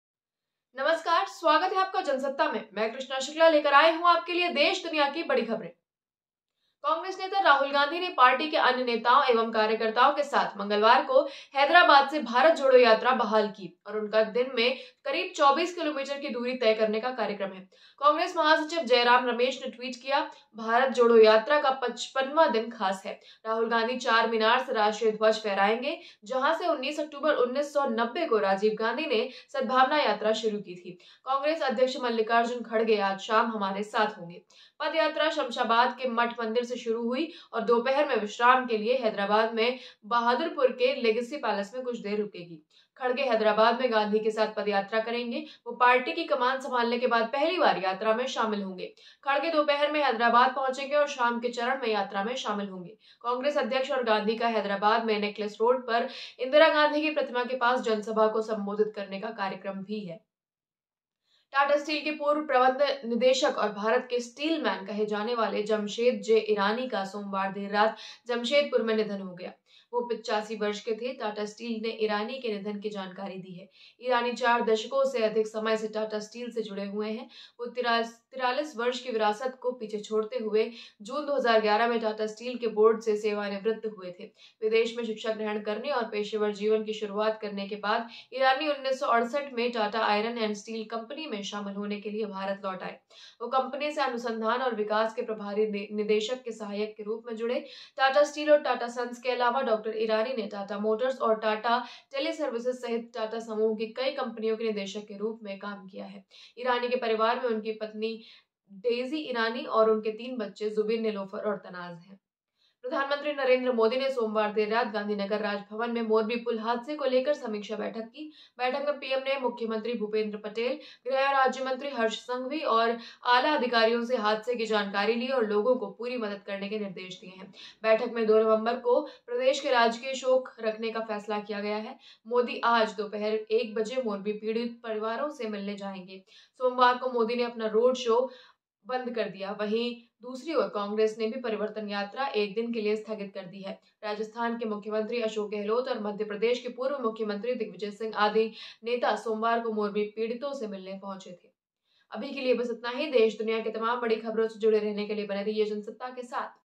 Bharat Jodo Yatra: Hyderabad से बहाल हुई, Rahul संग नज़र आएंगे Kharge, Jamshed J Irani का निधन
Morning News

कांग्रेस नेता राहुल गांधी (Congress leader Rahul Gandhi) ने पार्टी के अन्य नेताओं एवं कार्यकर्ताओं के साथ मंगलवार को हैदराबाद (Hyderabad) से ‘भारत जोड़ो’ यात्रा (Bharat Jodo Yatra) बहाल की और उनका दिन में करीब 24 किलोमीटर की दूरी तय करने का कार्यक्रम है। कांग्रेस महासचिव जयराम रमेश (Jayram Ramesh) ने ट्वीट (Tweet) किया, ‘‘भारत जोड़ो यात्रा का 55वां दिन खास है। राहुल गांधी चारमीनार से राष्ट्रीय ध्वज फहराएंगे, जहां से 19 अक्टूबर 1990 को राजीव गांधी (Rajiv Gandhi) ने सद्भावना यात्रा शुरू की थी।’’ कांग्रेस अध्यक्ष मल्लिकार्जुन खरगे (Mallikarjun Kharge) आज शाम हमारे साथ होंगे।’’ पदयात्रा शमशाबाद के मठ मंदिर से शुरू हुई और दोपहर में विश्राम के लिए हैदराबाद के बहादुरपुर के ‘लेगेसी पैलेस’ में कुछ देर रुकेगी। खरगे हैदराबाद में गांधी के साथ पदयात्रा करेंगे। वह पार्टी की कमान संभालने के बाद पहली बार यात्रा में शामिल होंगे। खरगे दोपहर में हैदराबाद पहुंचेंगे और शाम के चरण में यात्रा में शामिल होंगे। कांग्रेस अध्यक्ष और गांधी का हैदराबाद में नेकलेस रोड पर इंदिरा गांधी की प्रतिमा के पास जनसभा को संबोधित करने का कार्यक्रम है। Jansatta is one of the leading hindi news channel, which provides all the latest and trending news of india and around the world.